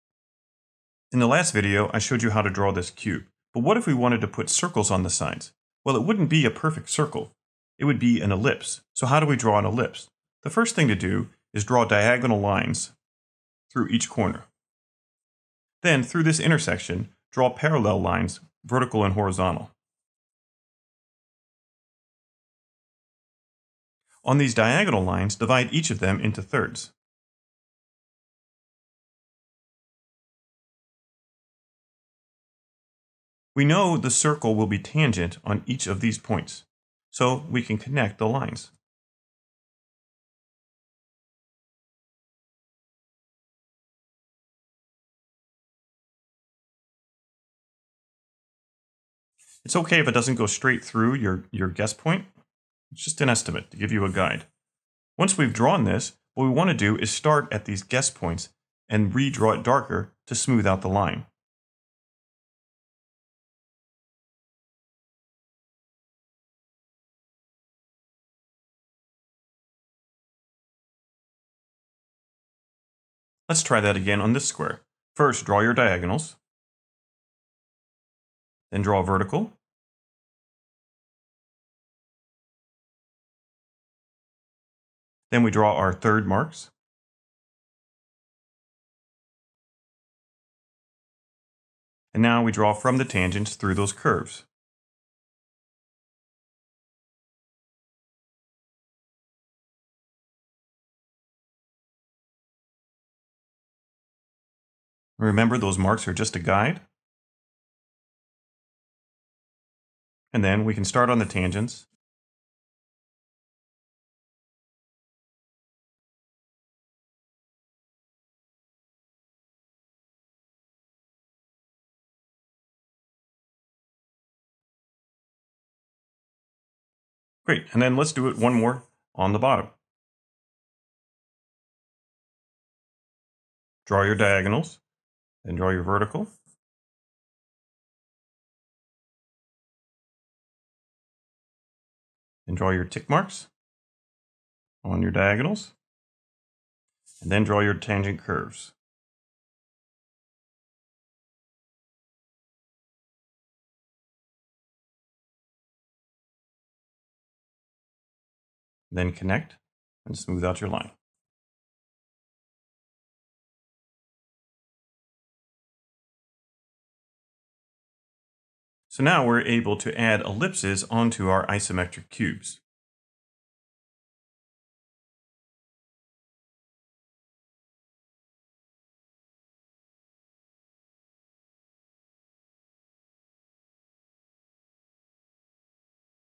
How to Draw an Isometric Circle Freehand
When making isometric ideation designs, everything won't be a square. You have to draw circles too but circles in isometric perspective are not circles, they are ellipses.

Learn how to quickly draw fairly accurate freehand circles in isometric perspective.

Intro to Technical Sketching For Artists and Designers